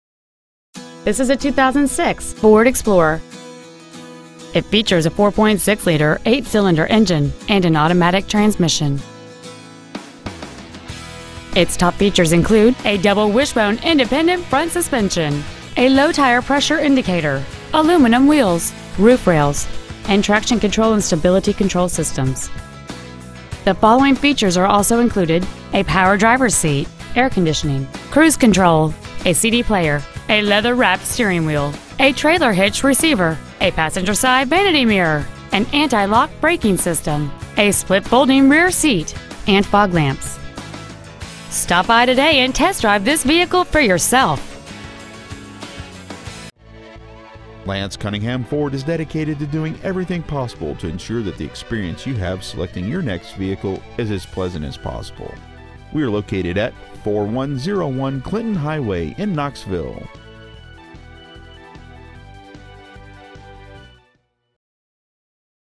Pre-Owned 2006 FORD EXPLORER Knoxville TN
Year : 2006
 Make : FORD
 Model : EXPLORER
 Engine : 4.6L V8 24V MPFI SOHC
 Trans . : 6-SPEED AUTOMATIC
 Exterior : RED
 Miles : 109,641

 Stock : P6645